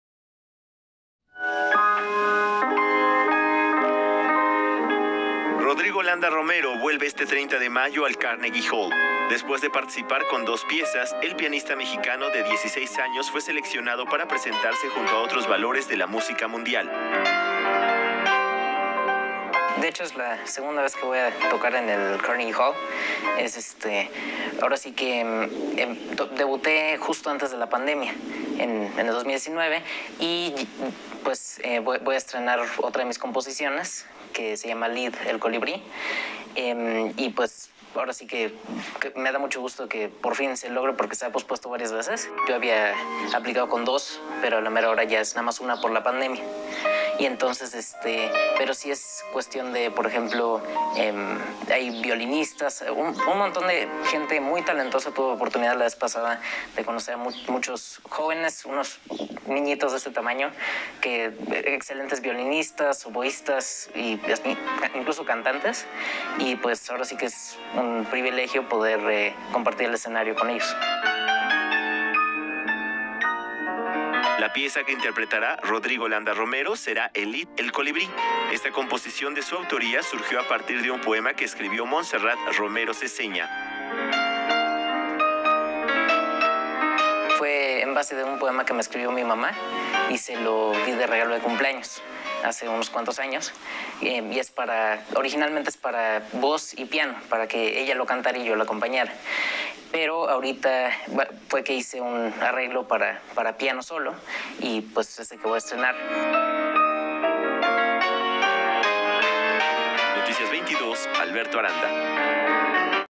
Entrevista con Rodrigo Landa-Romero por su concierto en el Carnegie Hall
Información transmitida el martes 17 de mayo del 2022 en Noticias22 por Canal 22